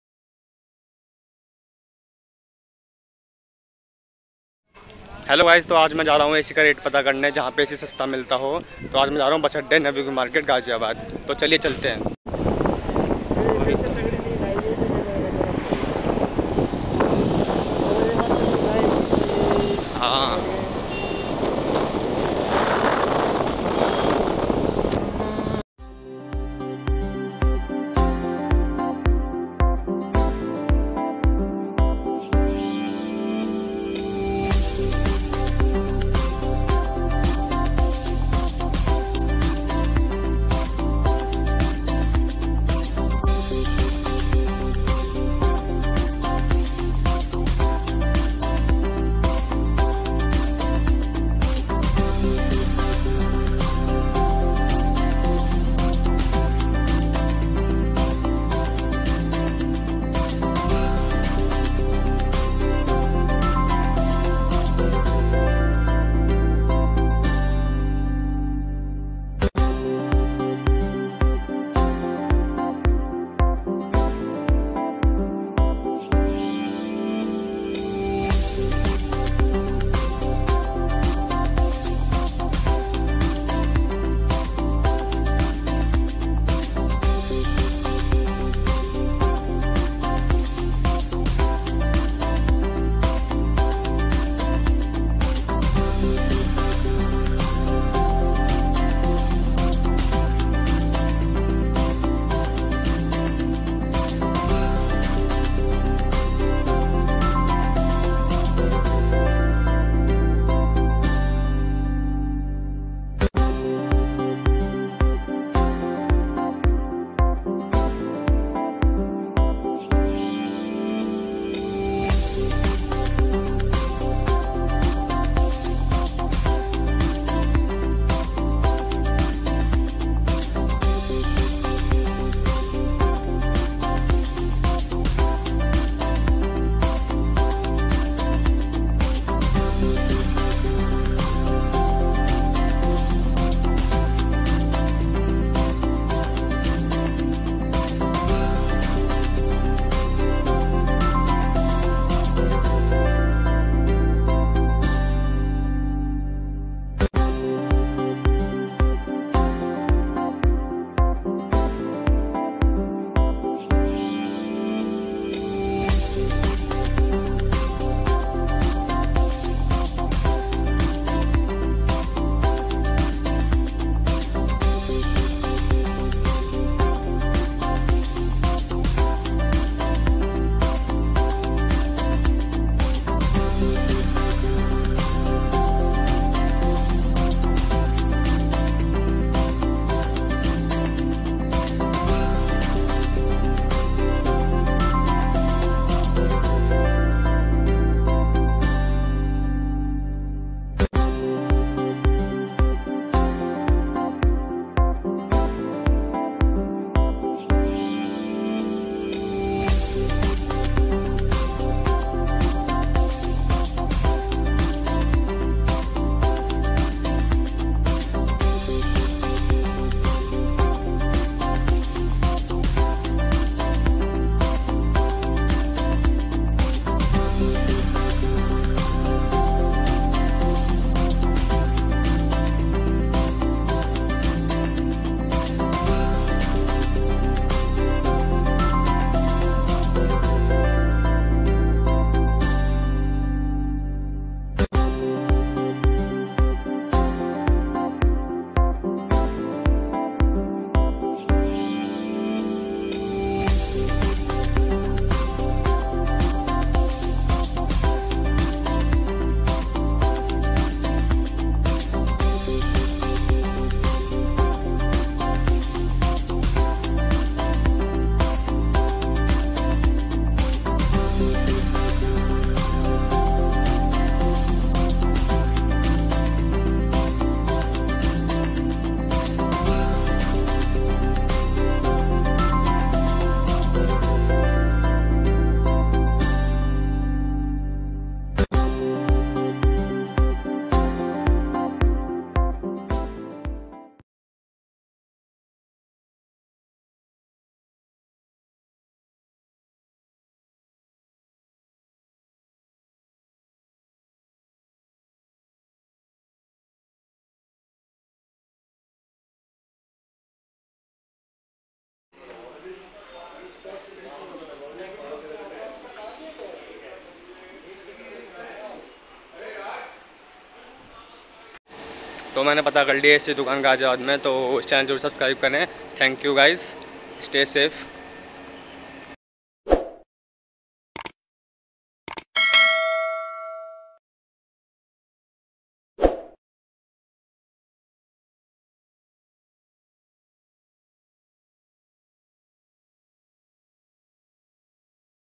Best AC Shop In Ghaziabad|Vlog|Cheapest AC Shop|LOCKDOWN में 40,000 का AC 23,500₹ में ले!!
||Vlog By Civil engineer Gyan baba||

Ghaziabad
Kavi Nagar
Raj nagar
Bus Adda
@civil engineer Gyan baba

Shop- Shiva Aircon Shop
Place-  Navyug market Ghaziabad